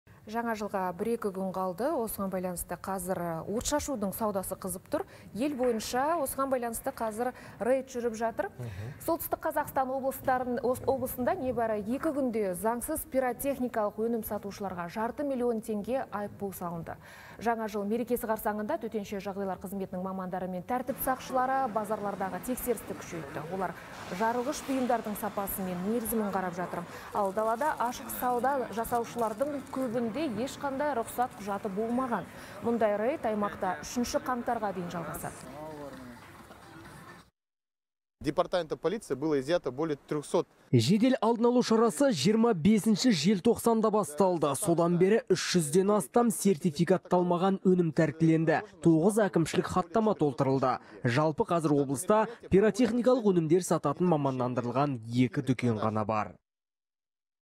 Заңсыз саудаланған пиротехника тәркіленді
Солтүстік Қазақстан облысында небәрі екі күнде заңсыз пиротехникалық өнім сатушыларға жарты миллион теңге айыппұл салынды.